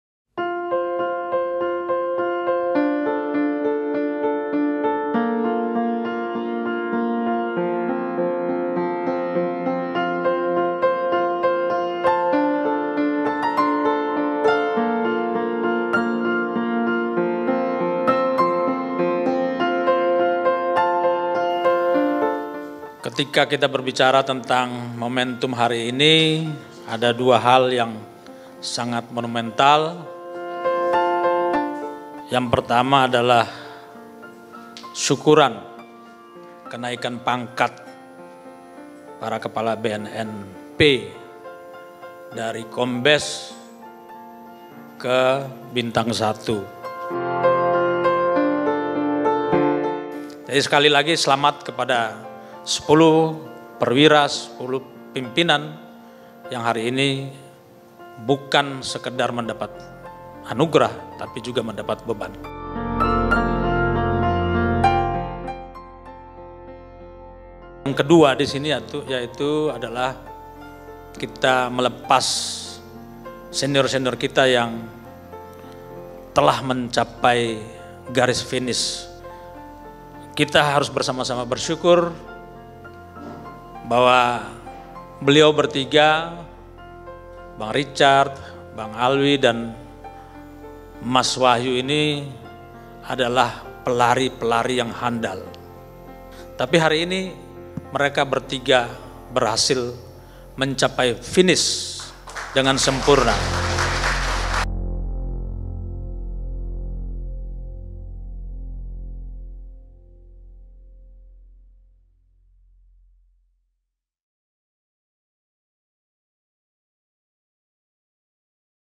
Pelepasan Jabatan Pimpinan Tinggi Madya Dan Kenaikan Pangkat Pimpinan Tinggi Pratama
Badan Narkotika Nasional (BNN) melaksanakan Pelepasan Pejabat Pimpinan Tinggi Madya dan Kenaikan Pangkat Pimpinan Tinggi Pratama, di Ruang Muh. Hatta, Gedung Tan Satrisna BNN, Cawang, Jakarta Timur, pada Jumat (30/8).

Terdapat tiga orang Pejabat Tinggi Madya yang memasuki masa purna tugas pada September 2024 mendatang, yaitu Deputi Pencegahan BNN RI, Dr. Drs. Richard M. Nainggolan, S.H., M.M., MBA, Kepala Pusat Laboratorium Narkotika BNN, Ir. Wahyu Widodo, dan Inspektur Wilayah I Inspektorat Utama BNN, Drs. Ahmad Alwi, M.M., CFRA. Sedangkan Anggota Polri penugasan BNN yang mendapatkan kenaikan pangkat berjumlah 10 orang, dengan pangkat Brigadir Jenderal Polisi.

Kepala BNN RI, Marthinus Hukom, S.I.K., M.Si., dalam sambutannya mengatakan bahwa Ia memaknai kenaikan pangkat dalam dua dimensi berbeda yang harus dipahami. Dimensi pertama adalah sosiokultural, yaitu kenaikan pangkat memberikan perubahan pada individu dan bagaimana masyarakat melihat saat seseorang diberikan level sosial yang lebih tinggi.

“Pangkat adalah kehormatan. Pangkat adalah level sosial, maka Kita harus berperilaku sesuai dengan level sosial Kita dan pangkat Kita," ujar Marthinus Hukom.

Dimensi lainnya adalah sosiostruktural, artinya kenaikan pangkat merupakan tanggung jawab yang diberikan oleh negara serta mampu memberikan kepercayaan kepada masyarakat saat melaksanakan tugas dalam menghadapi berbagai tantangan internal maupun eksternal.

“Pangkat adalah kebanggaan maka Kita harus bersyukur atas anugerah yang diberikan oleh Tuhan, kepercayaan yang diberikan oleh negara sehingga kita dapat mencapai titik tersebut," ungkap Kepala BNN RI lebih lanjut.

Usai kegiatan, Kepala BNN RI menyampaikan ucapan selamat dan memberikan cinderamata kepada Pejabat Tinggi Madya yang memasuki masa purna tugas dan Pejabat Tinggi Pratama yang mendapatkan kenaikan pangkat.

BIRO HUMAS DAN PROTOKOL BNN